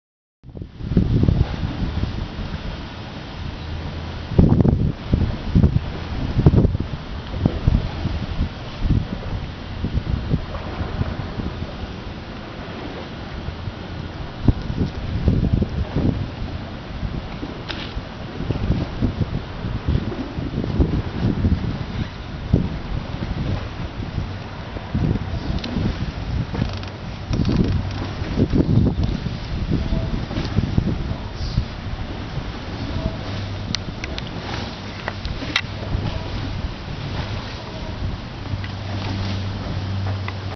Old Town Alexandria / Potomac River / Woodrow Wilson Bridge
Begins with North - South sweep of Potomac River as seen from terminus of Alexander St. in Old Town Alexandria, VA. Ends with view of new Woodrow Wilson Bridge under construction. Just north of Jones Point Park. Bolling AFB & Naval Research Lab across river.